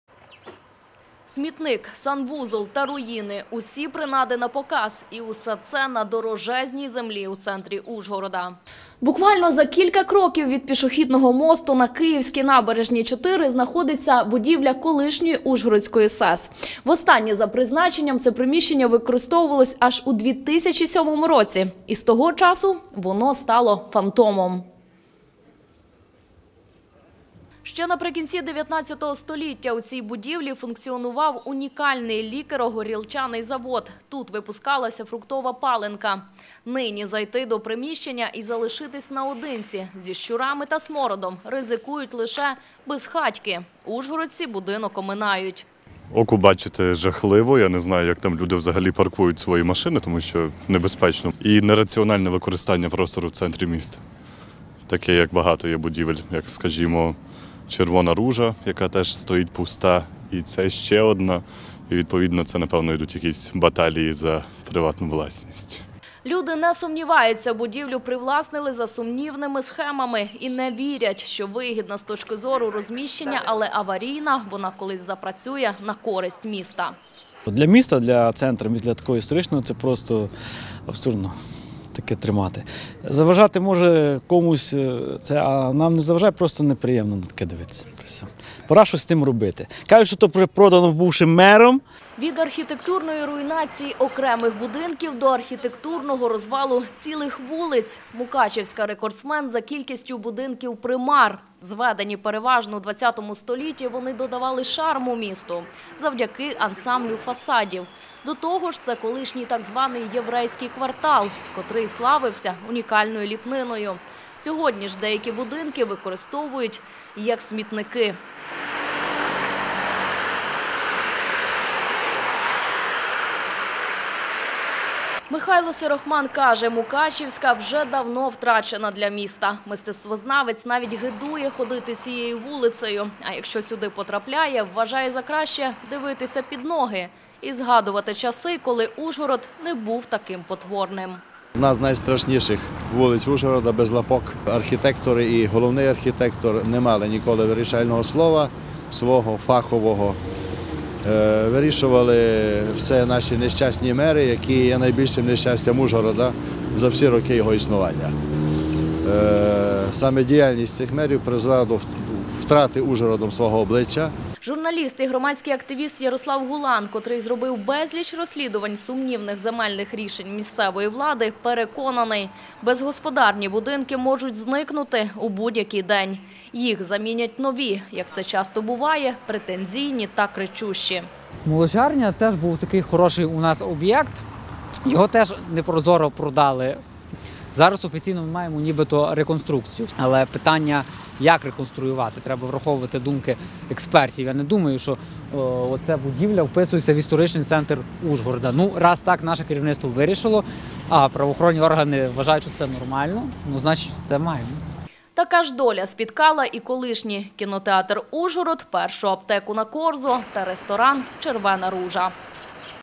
Будинки-примари
Будинки-примари! В центральній частині Ужгорода вже роками  пусткою стоять кілька будівель. Кожна колись мала своє призначення - була хімчисткою, аптечним складом, санстанцією. Тепер – займає цінні «квадратні» метри землі та псує архітектурне обличчя міста. Вигляд деяких - просто жалюгідний. Вікна давно вибиті, всередині – гори сміття та ганчір’я, і притулок для безхатьків. Олена Остапчук покаже найбільш показові приклади.